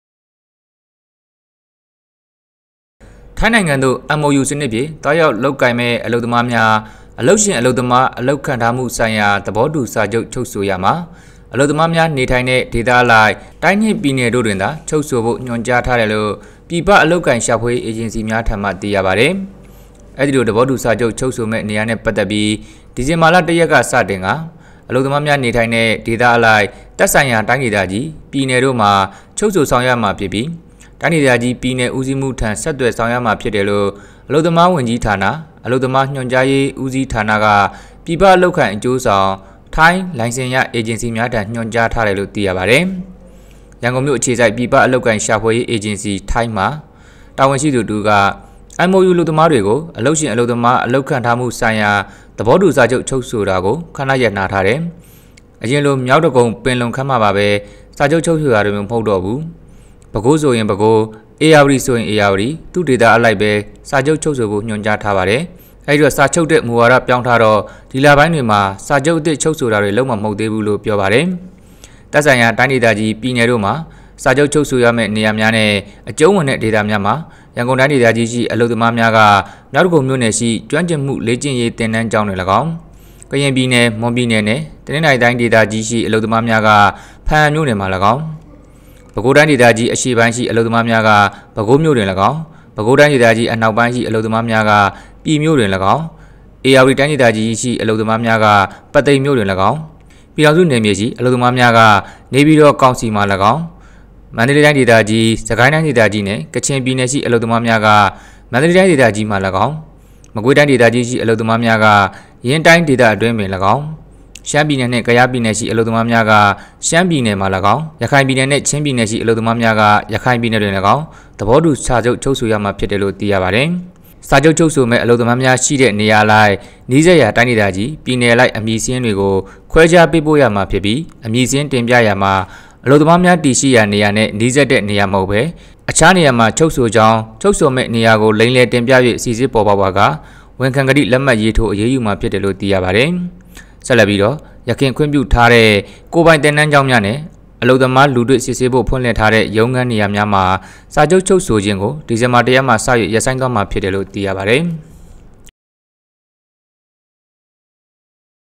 ထိုင်းနိုင်ငံသို့MOUစနစ်ဖြင့်သွားရောက်လုပ်ကိုင်မည့်အလုပ်သမားများ...
ထိုင်းနိုင်ငံသို့MOUစနစ်ဖြင့်သွားရောက်လုပ်ကိုင်မည့်အလုပ်သမားများ အလုပ်ရှင်၊အလုပ်သမားအလုပ်ခန့်ထားမှုဆိုင်ရာသဘောတူ စာချုပ်ချုပ်ဆိုရာတွင် အလုပ်သမားများနေထိုင်သည့် ဒေသအလိုက် တိုင်းနှင့် ပြည်နယ်တို့၌သာချုပ်ဆိုရန် ညွှန်ကြား

ထိုင်းနိုင်ငံသို့MOUစနစ်ဖြင့်သွားရောက်လုပ်ကိုင်မည့် အလုပ်သမားများ အလုပ်ရှင်၊ အလုပ်သမား အလုပ်ခန့်ထားမှု ဆိုင်ရာ သဘောတူစာချုပ် ချုပ်ဆိုရာတွင်အလုပ်သမားများနေထိုင်သည့်ဒေသအလိုက်တိုင်းနှင့်ပြည်နယ်တို့တွင်သာချုပ်ဆိုရန်ညွှန်ကြားထားကြောင်းပြည်ပအလုပ်အကိုင် ရှာဖွေရေး အေဂျင်စီများထံမှ သိရသည်။